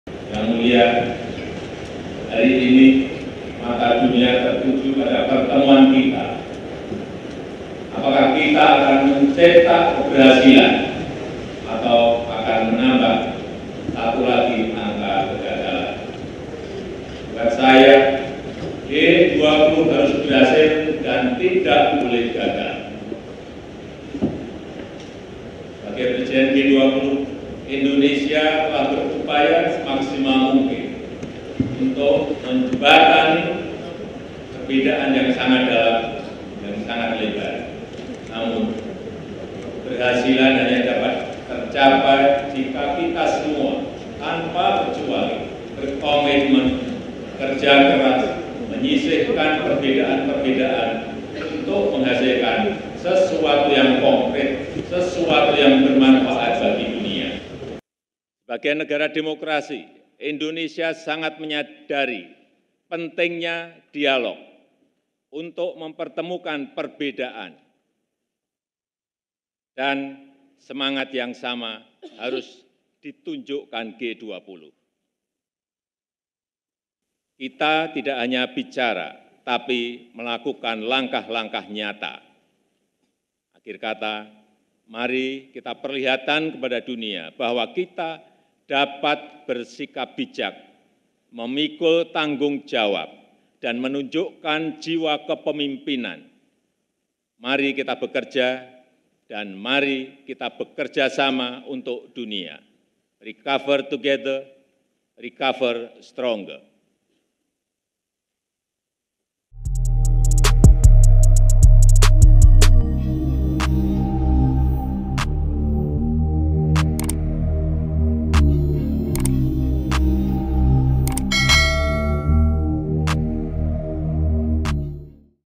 Jokowi Tegaskan KTT G20 Harus Berhasil, Tak Boleh Gagal
Presiden Joko Widodo berharap pelaksanaan KTT G20 tidak boleh mengalami kegagalan.

Sebab, Jokowi menilai bahwa saat ini G20 menjadi pusat perhatian dunia.

Hal itu disampaikan Jokowi dalam pidato pembukaan KTT G20, Selasa (15/11/2022).

Jokowi menjelaskan, sebagai Presidensi G20 tahun ini, Indonesia telah berupaya semaksimal mungkin untuk menjembatani perbedaan yang sangat dalam dan lebar.

Namun, keberhasilan hanya akan dicapai jika semua pihak, tanpa terkecuali, berkomitmen, bekerja kerjas, menyingkirkan perbedaan-perbedaan untuk menghasilkan sesuatu yang konkret dan bermanfaat bagi dunia.

Produser: Sherly Puspita